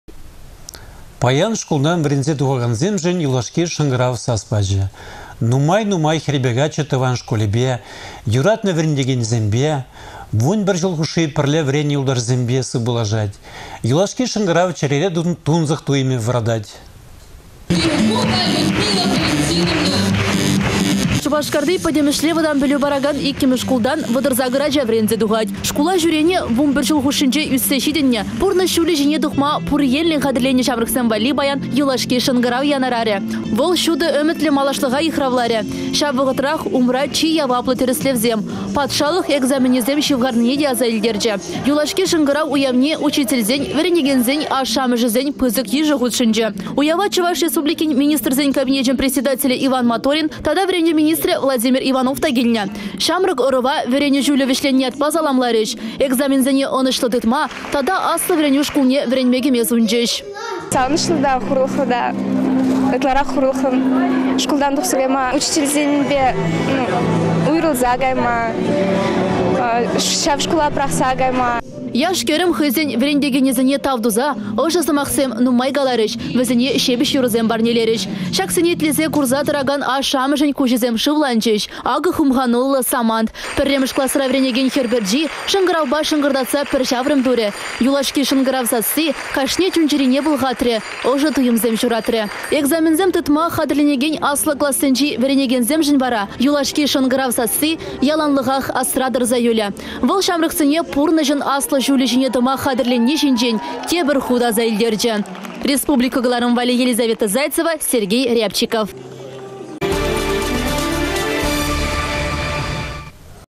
Юлашки шăнкăрав çамрăксене ырă çул пиллет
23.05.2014
Шкултан вĕренсе тухакансемшĕн юлашки шăнкăрав сас пачĕ. Нумай-нумай хĕрпе каччă тăван шкулĕпе, юратнă вĕрентекенĕсемпе, вун пĕр çул хушши пĕрле вĕреннĕ юлташĕсемпе сыв пулашать. Юлашки шăнкăрав чĕрере тунсăх туйăмĕ вăратать.